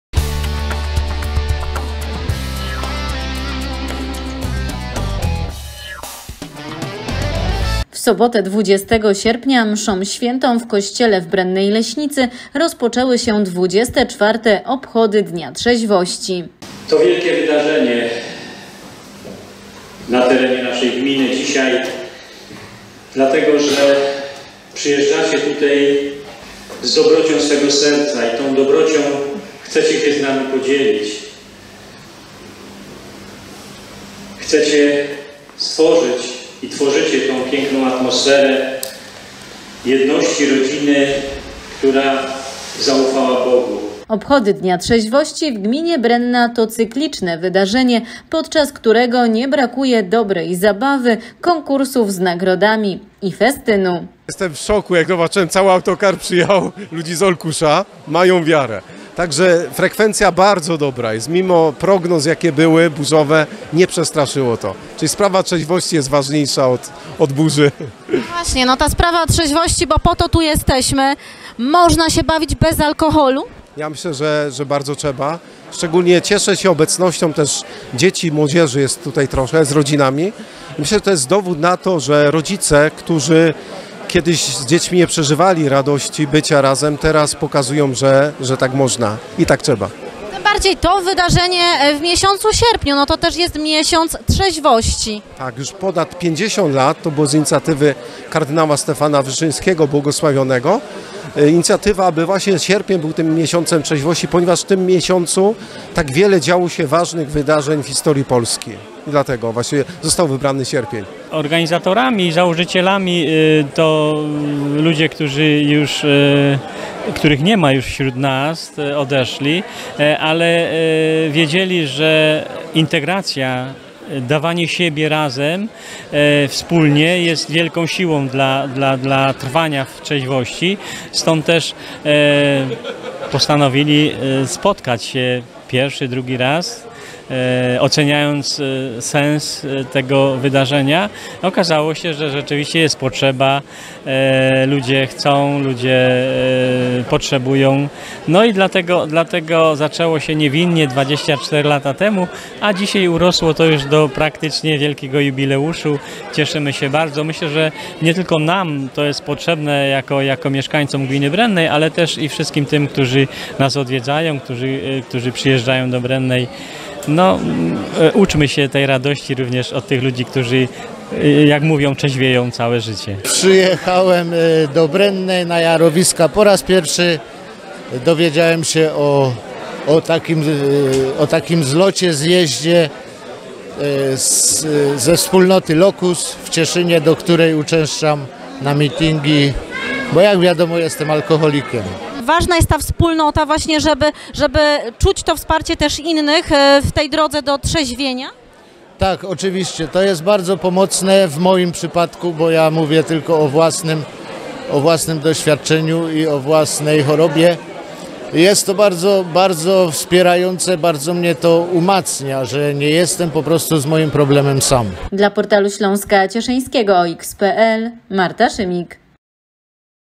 W Brennej obchodzili dzień trzeźwości
Wczoraj (20.08) w Brennej Leśnicy „Jarowiska” Mszą św. rozpoczęły się XXIV obchody Dnia Trzeźwości. Organizatorem tych cyklicznych spotkań jest Gmina Brenna oraz Duszpasterz Trzeźwości Diecezji Bielsko-Żywieckiej Ks. Piotr Leśniak.